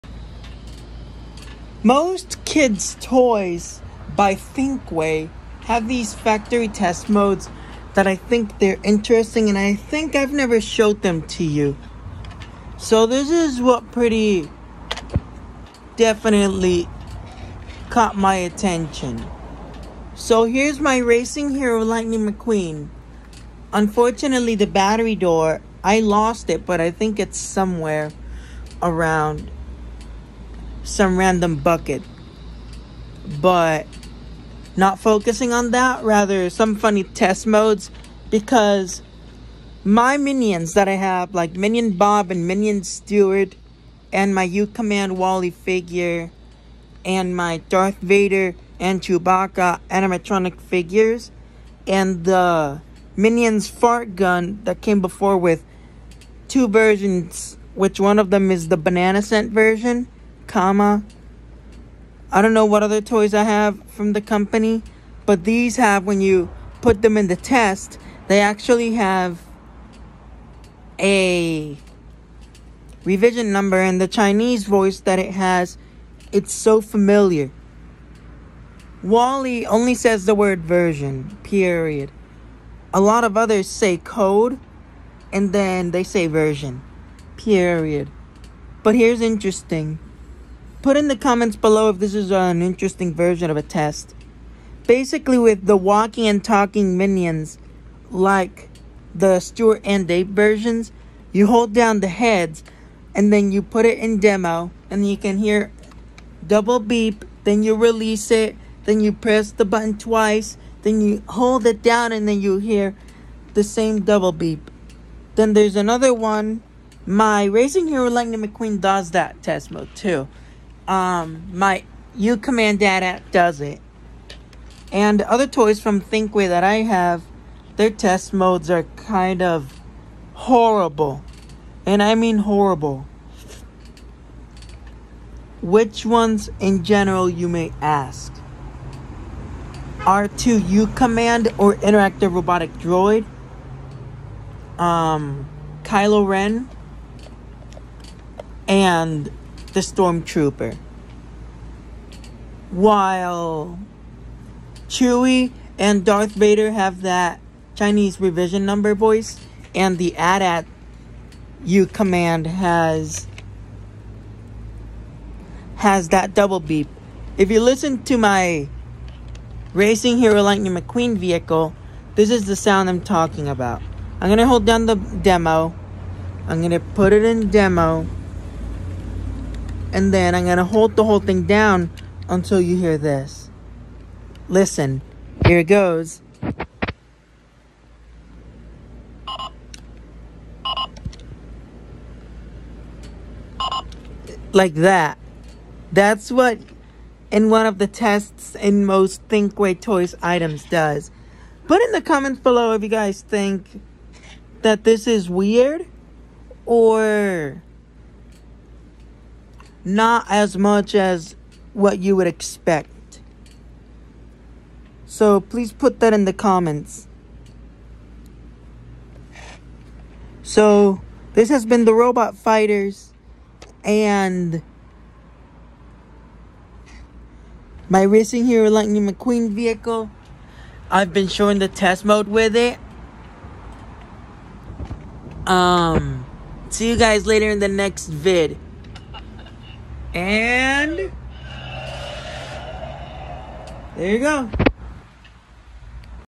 Put in the comments below if you think that some factory test modes are strange.
Just like the title says, let me know if these test modes are weird. This is particularly for thinkway toys. For some Youtubers that own toys like this, let me know if some of their test modes are some randomly weird or they’re pretty normal. ￼